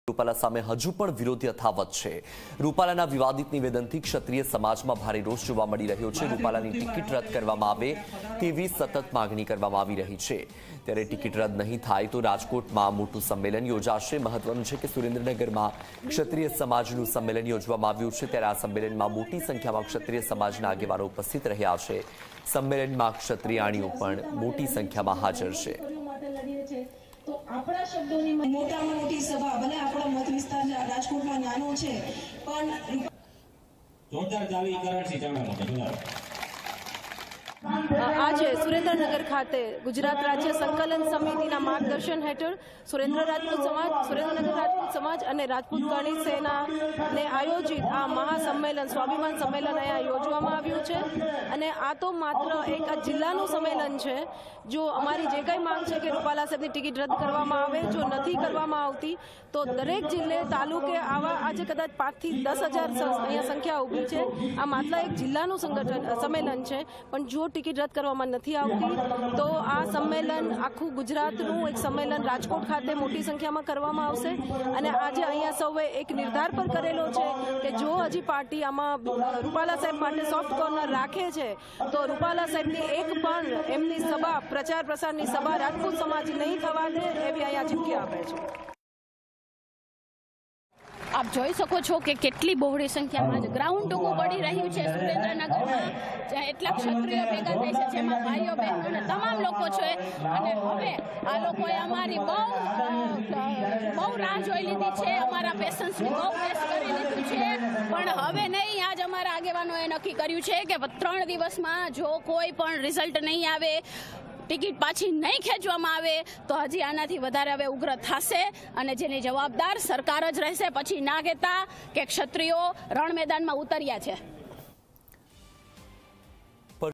Parshotam Rupala સામે હજુ પણ વિરોધ યથાવત, Ticket રદ નહીં થાય તો Rajkotમાં યોજાશે મોટું સંમેલન
પરશોત્તમ રૂપાલા સામે હજુ પણ વિરોધ યથાવત  ---

રૂપાલાના વિવાદિત નિવેદનથી ક્ષત્રિય સમાજમાં ભારે રોષ   ---

રૂપાલાની ટિકિટ રદ કરવામાં આવે તેવી માગઃ તૃપ્તિબા   ---

ટિકિટ રદ નહીં થાય તો રાજકોટમાં યોજાશે મોટું સંમેલન

મહત્વનું છેકે સુરેન્દ્રનગરમાં ક્ષત્રિય સમાજનું સંમેલન યોજવામાં આવ્યું છે... ત્યારે આ સંમેલનમાં મોટી સંખ્યામાં ક્ષત્રિય સમાજના આગેવાનો  ઉપસ્થિત રહ્યા છે... સંમેલનમાં ક્ષત્રાણિયો પણ મોટી સંખ્યામાં હાજર રહી છે..